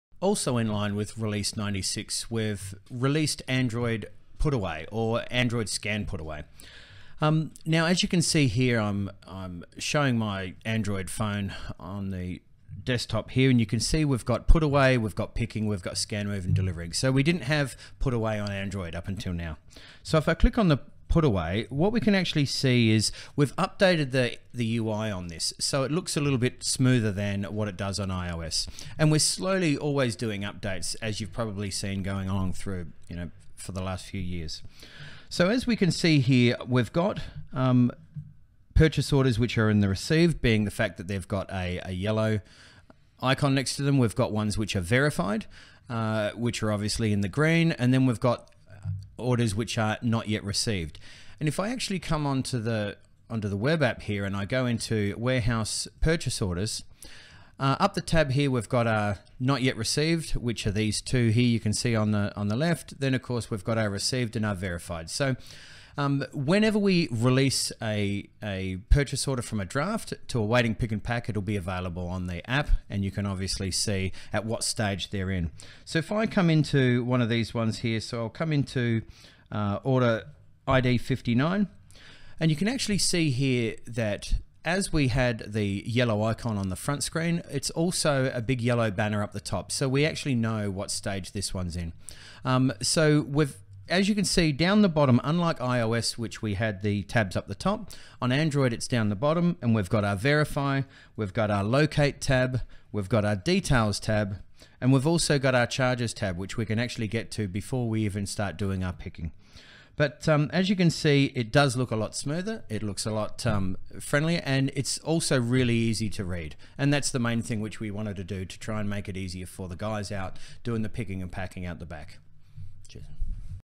Product Update - Android Barcode Scan Putaway
By rolling out to Android, we've massively increased the number of devices which can be used with the CartonCloud Warehouse Management System. From low-cost consumer-grade Android Phones to rugged enterprise-grade freezer-capable devices from manufacturers such as Zebra, Motorola and Cipherlab.